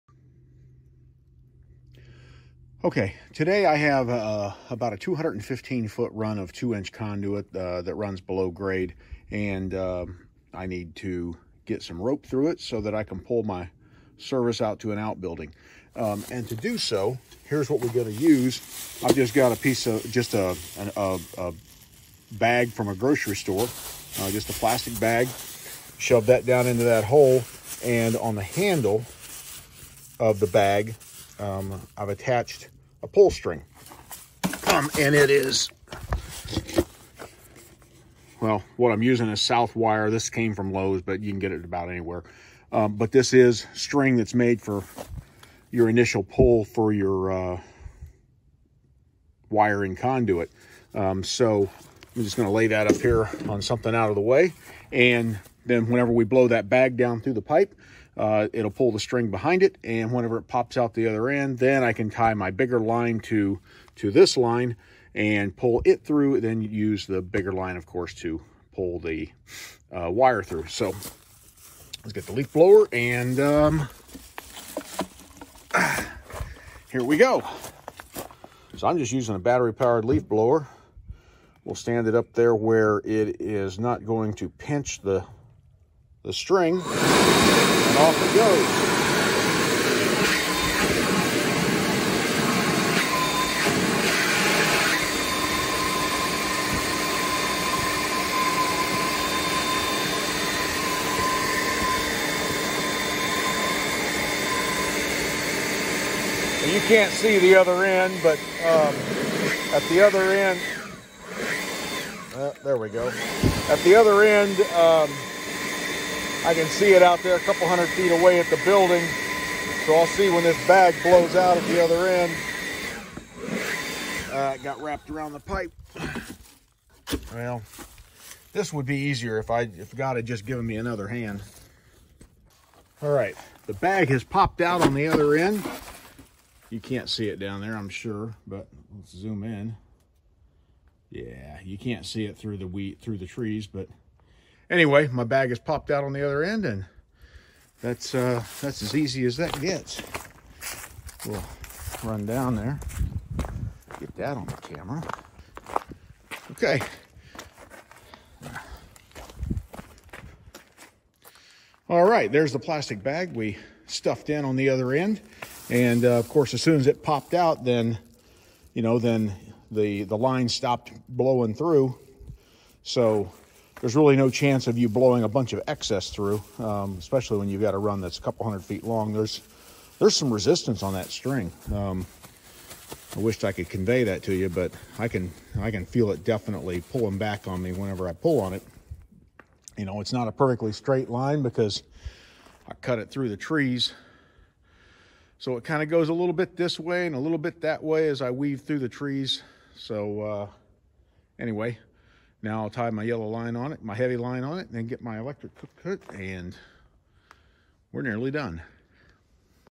Blowing line through conduit with a leaf blower.
Using a grocery sack, string, and a battery powered leaf blower to blow string through a 215 feet long conduit run underground.